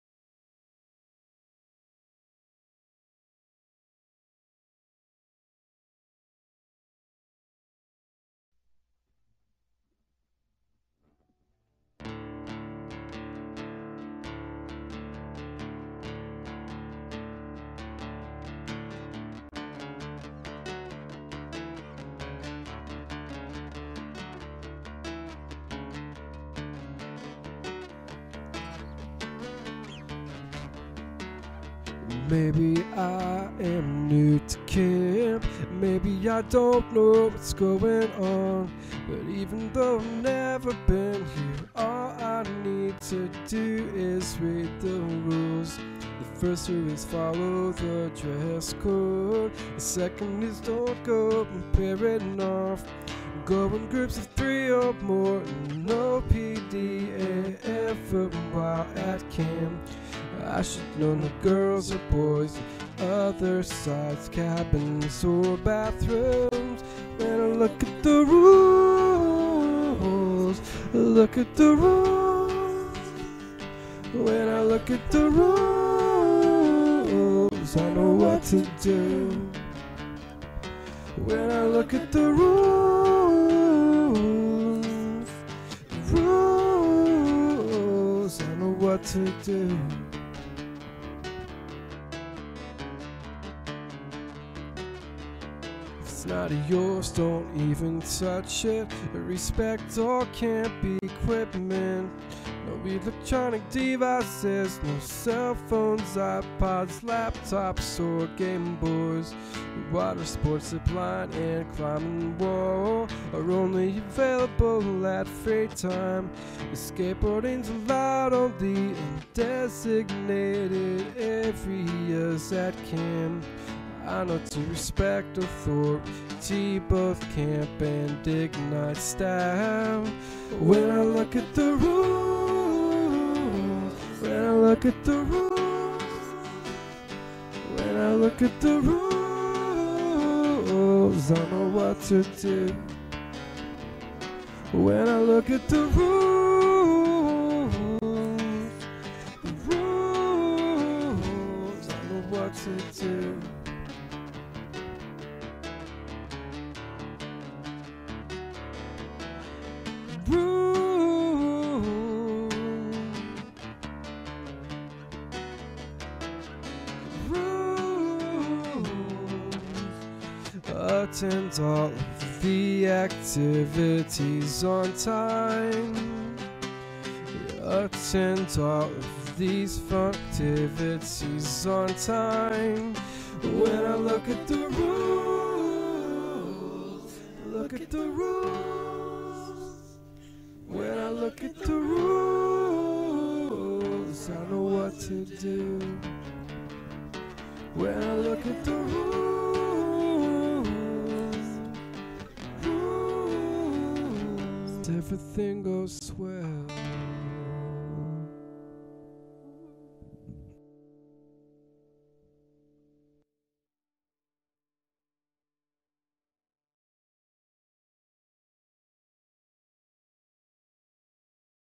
iGNITE Rules Video 2006
Rules for iGNITE Summer Camp 2006 done to the music of Switchfoot!

Lead Vocals: Bryce Langebartels (sorry David)
Background Vocals: Cammi Comer, Garrett Lagebartels, Clay Langebartels, Kristin Langebartels.